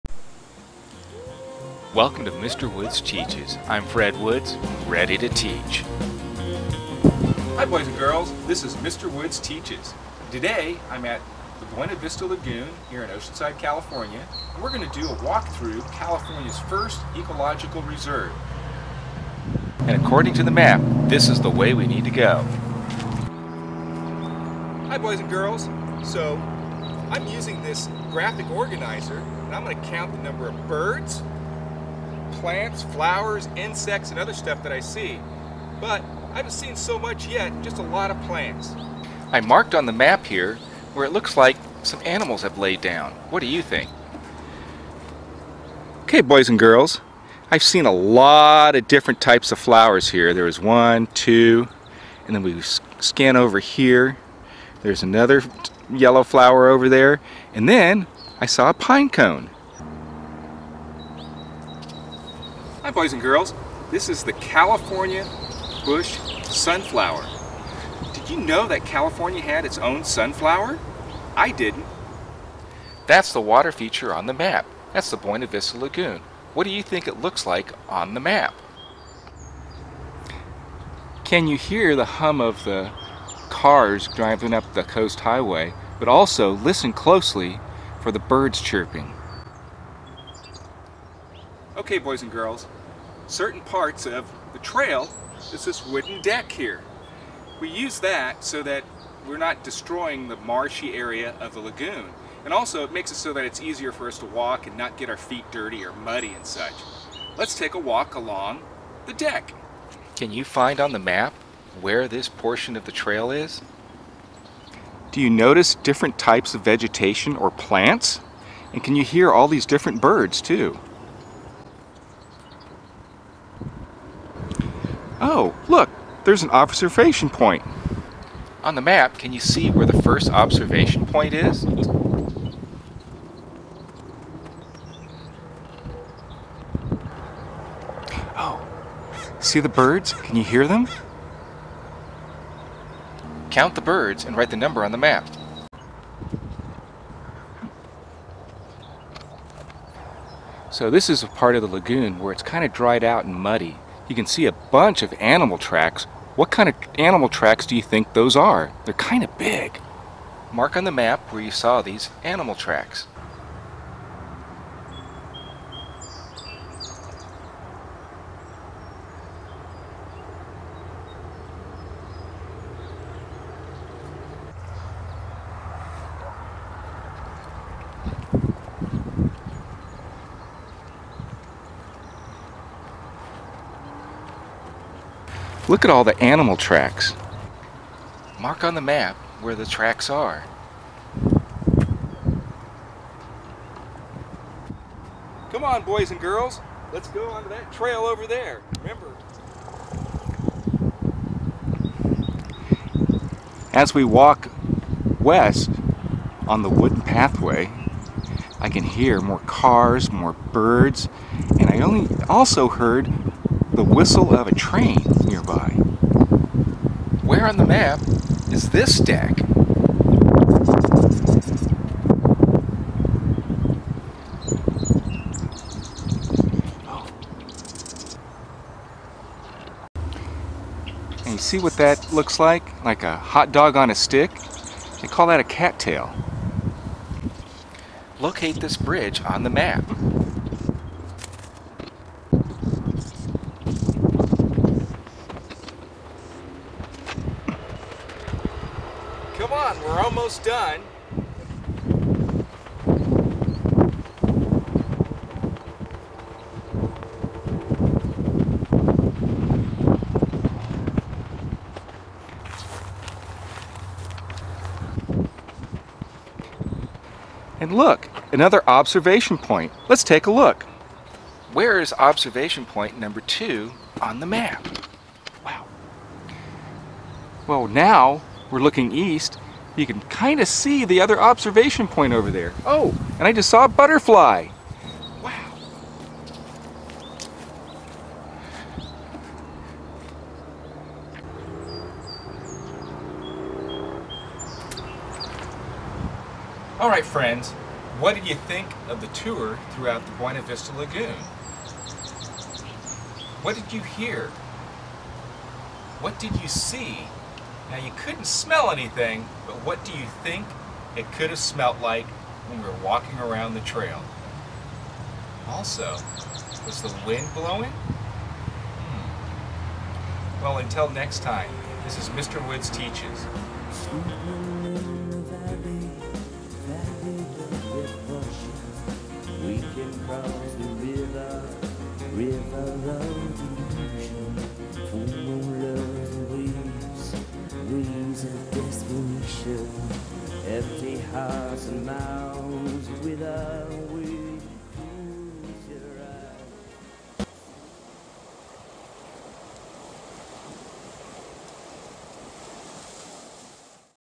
Mr Woods Teaches | Buena Vista Lagoon Field Trip | Geography
This is a quick video tour of the Buena Vista Lagoon in Oceanside, CA, located at 2202 S. Coast Highway, Oceanside, CA 92054,

The lesson was originally created for 2nd-grade but can be differentiated for all elementary grades.

Standards
NGSS.2-LS4-1. Make observations of plants and animals to compare the diversity of life in different habitats [Clarification Statement: Emphasis is on the diversity of living things in each of a variety of different habitats.] [Assessment Boundary: Assessment does not include specific animal and plant names in specific habitats.].

 ELD Standards: Part I: Interacting in Meaningful Ways
A. Collaborative (engagement in dialogue with others)
1. Exchanging information and ideas via oral communication and conversations
2. Interacting via written English (print and multimedia)

Learning Goals:
Using their five senses, the students will observe the diverse animal and plant life of the wetland habitat. Students will record their observations on a graphic organizer. Through writing and oral collaboration, the students will compare the similarities and differences of their habitat observations.

What to do using the graphic organizer: While walking the trail observe and note sea birds, ducks, and other birds. You note what vegetation (plants) that you see, such as the reeds were extra tall this year. Keep counts of the different types of birds, flowers, insects, reptiles, and trees. Draw pictures of what you saw, smelt, heard, or touched.

How else can you extend learning? Send me feedback.

Music/SoundFX

Videography/Photography